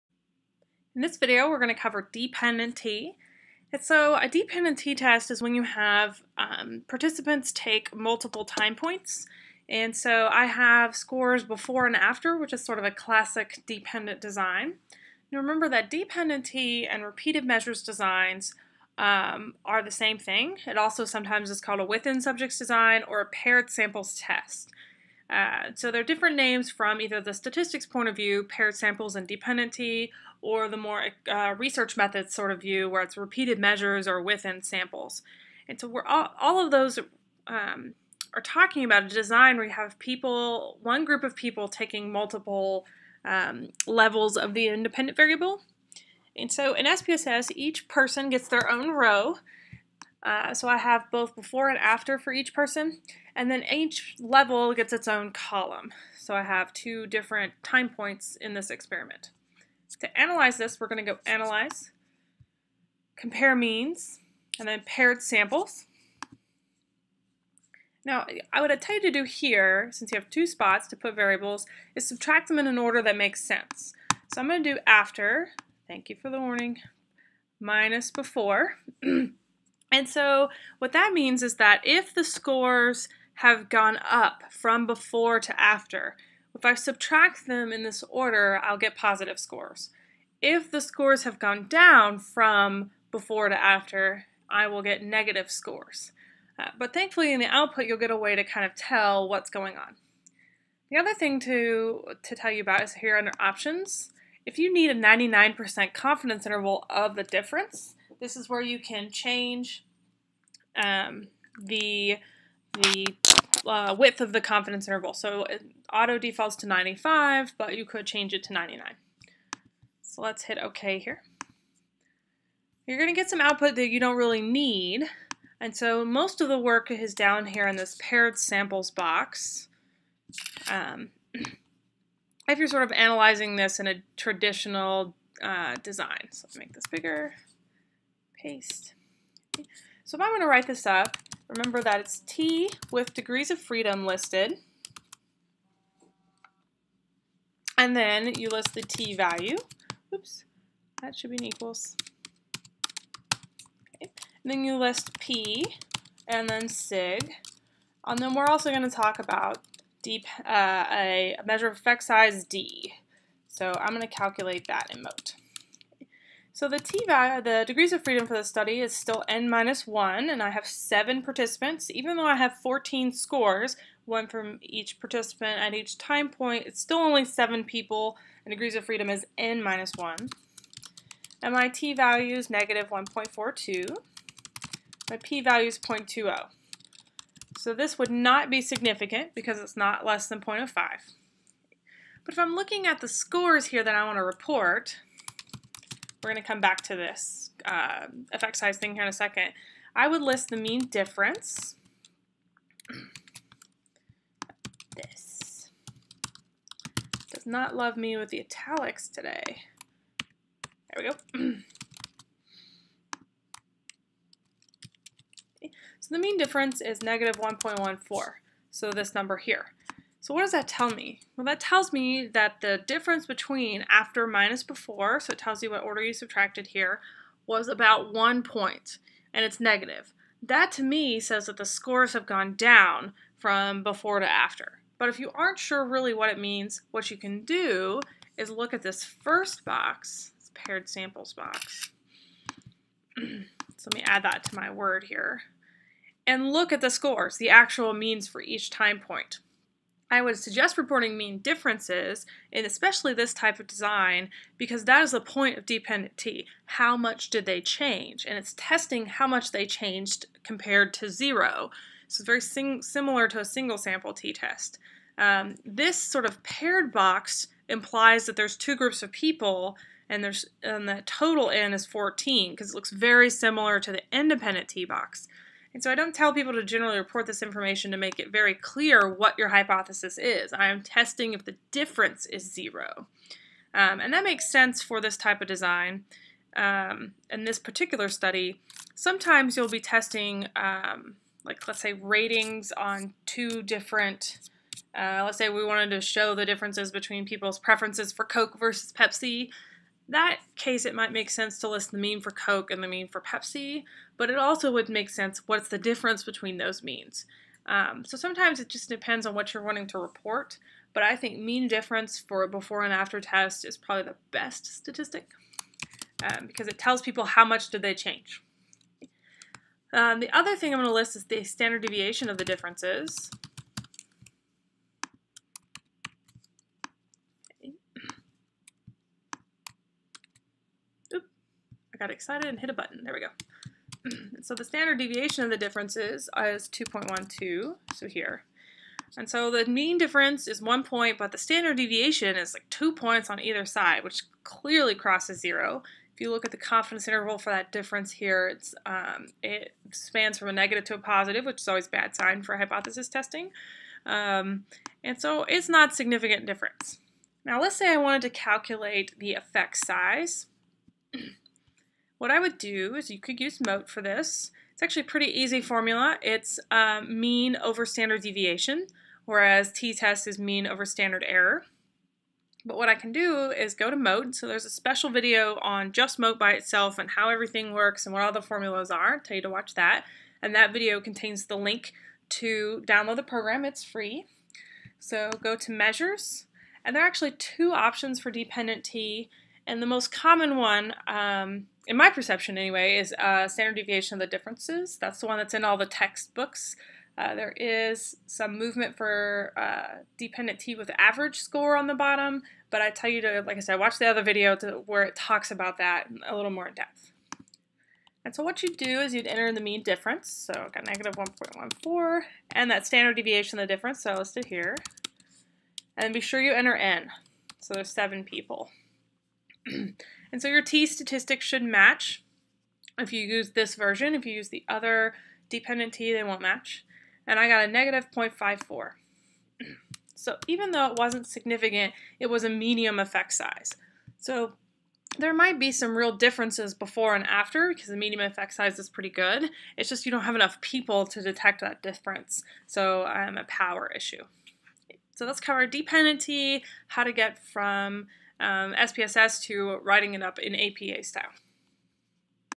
SPSS - Dependent (Paired) t-test Example
Lecturer: Dr. Erin M. Buchanan
Missouri State University

This video covers how to run a dependent t-test in SPSS, use MOTE to calculate the different types of effect sizes, and report those findings in APA style.

Lecture materials and assignments available at statisticsofdoom.com.